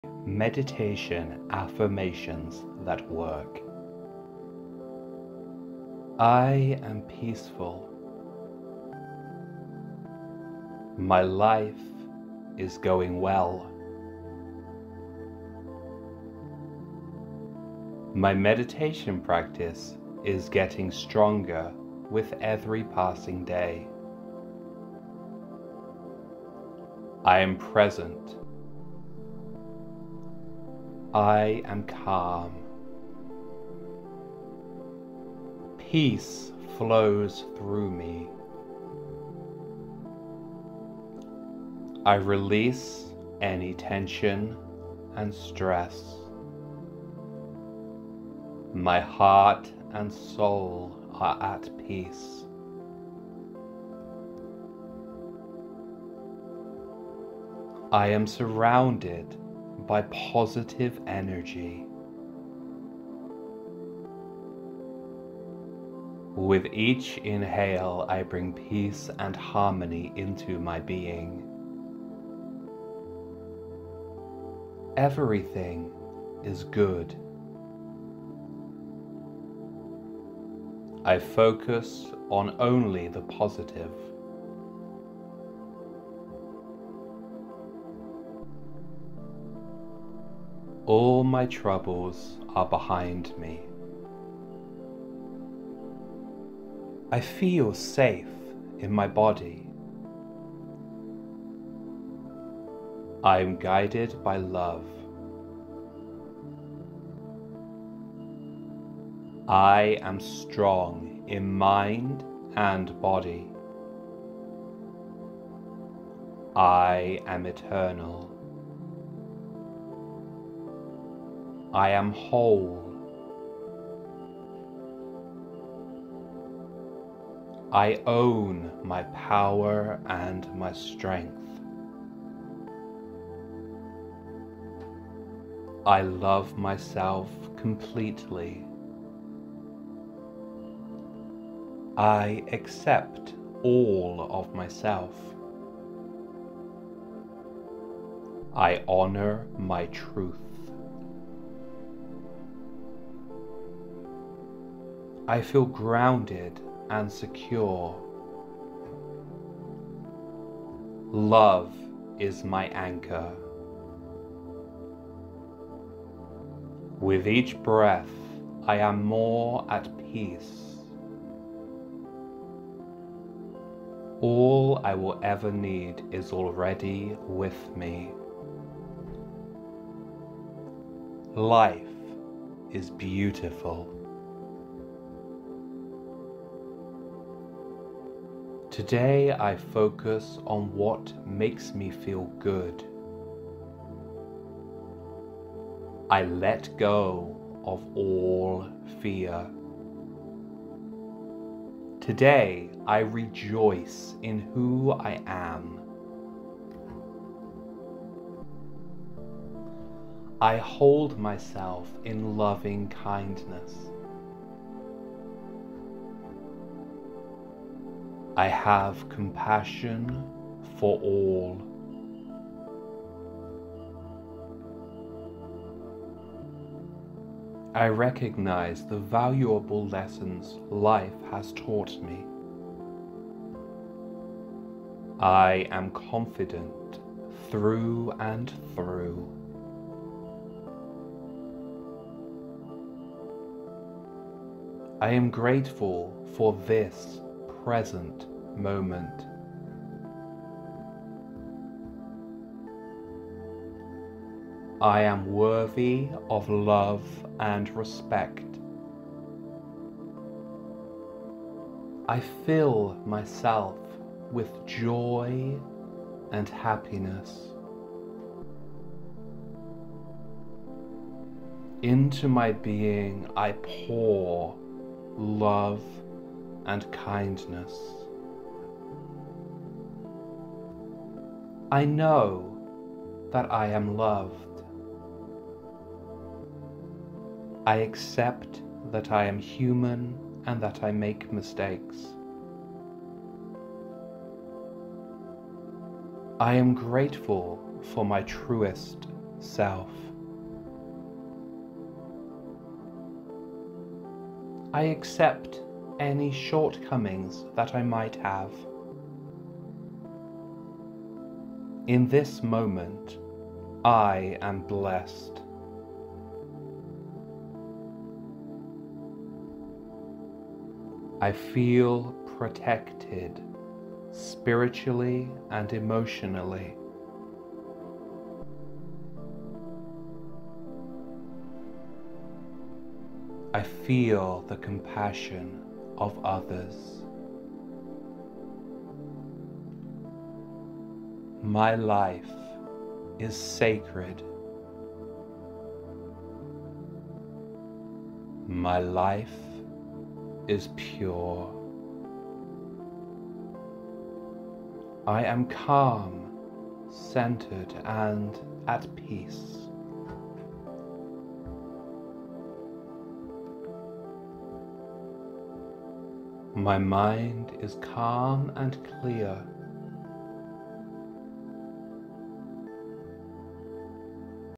50 Meditation Affirmations That Actually Work
Enjoy this list of 50 meditation affirmations that work

I am peaceful
My life is going well
My meditation practice is getting stronger with every passing day
I am present
I am calm
Peace flows through me
I release any tension and stress
My heart and soul are at peace
I am surrounded by positive energy
With each inhale I bring peace and harmony into my being
Everything is good
I focus on only the positive
All my troubles are behind me
I feel safe in my body
I am guided by love
I am strong in mind and body
I am eternal
I am whole
I own my power and my strength
I love myself completely
I accept all of myself
I honour my truth
I feel grounded and secure
Love is my anchor
With each breath I am more at peace
All I will ever need is already with me
Today I focus on what makes me feel good
I let go of all fear
Today I rejoice in who I am
I hold myself in loving kindness
I have compassion for all
I am confident through and through
I am grateful for this present moment
I an worthy of love and repect
I fill myself with joy and happiness
In to my being I pour love and kindness
I know that I am loved
I accept that I am human and that I make mistakes
I am grateful for my truest self
I accept and shortcomings that I might have
In this moment I am blessed
I feel protected spiritually and emotionally
I feel the compassion of others
My life is sacred
My life is pure
I am calm, centred and at peace
My mind is calm and clear

Which of these is your favorite meditation affirmation?